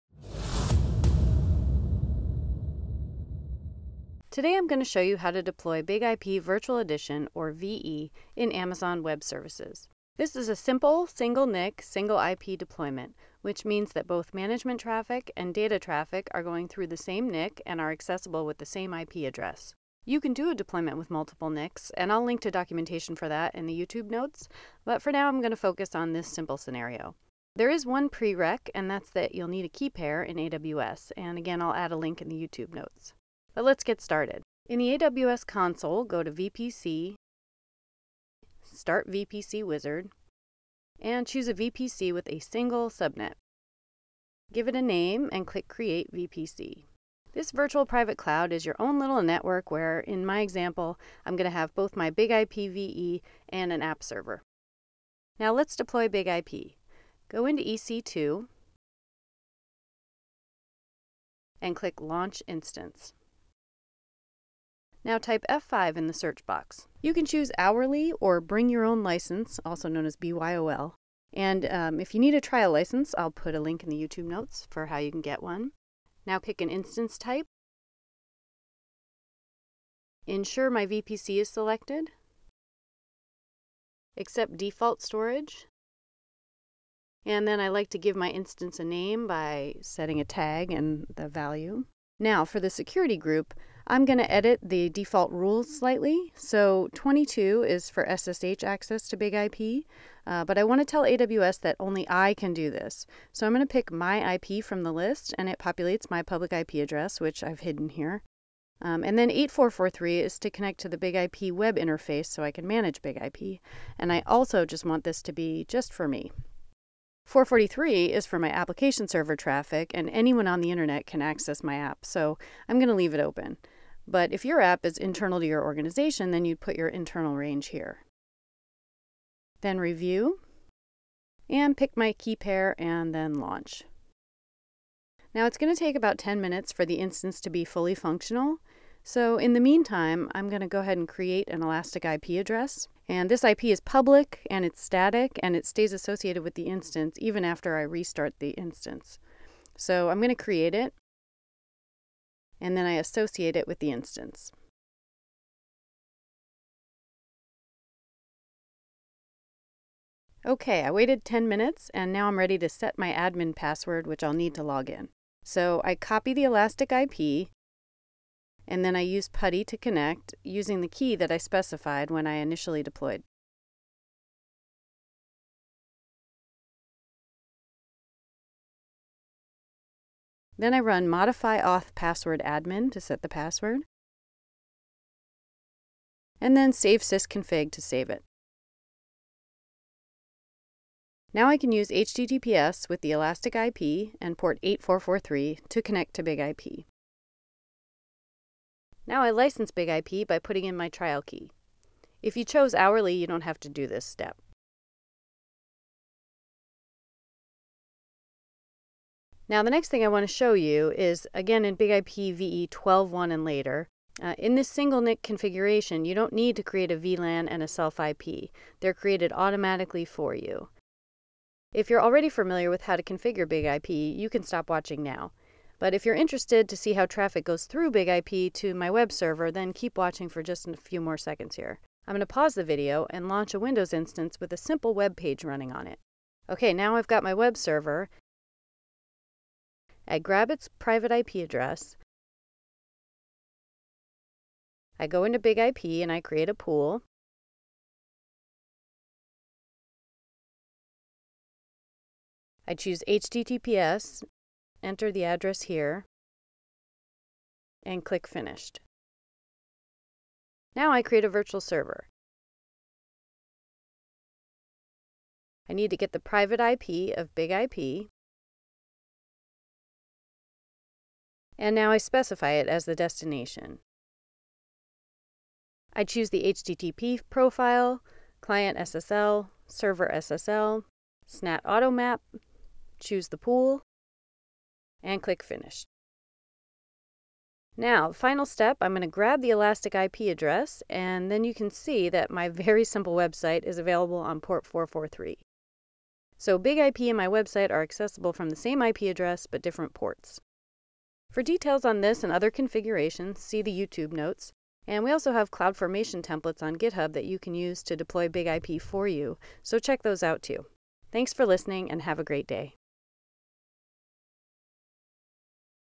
[Archived - See description for updated information] BIG-IP VE in Amazon Web Services (AWS)
This video provides a step-by-step guide on deploying BIG-IP Virtual Edition (VE) in Amazon Web Services (AWS). It covers a simple single NIC and single IP deployment, where both management and data traffic are routed through the same network interface. Viewers will learn the essential setup procedures and configurations necessary for successful deployment in the AWS environment.